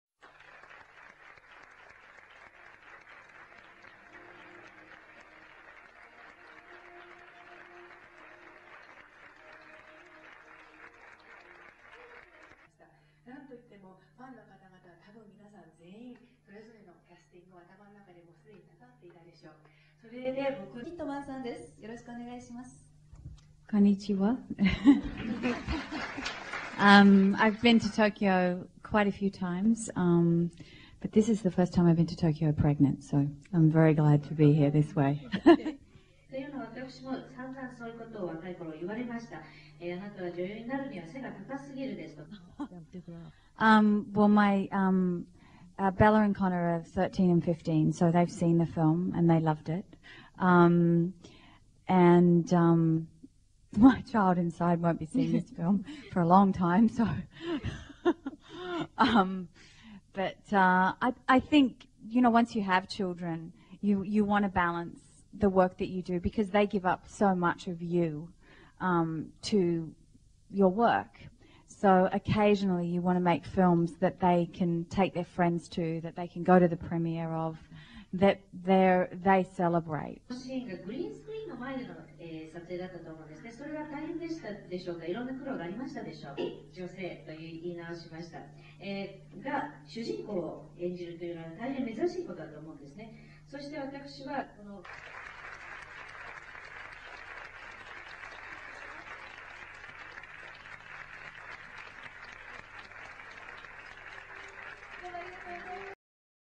Dakota Blue Richards | 2008 'The Golden Compass' Tokyo Press Conference
Dakota Blue Richards, Nicole Kidman and Chris Weitz at 'The Golden Compass' Tokyo Press Conference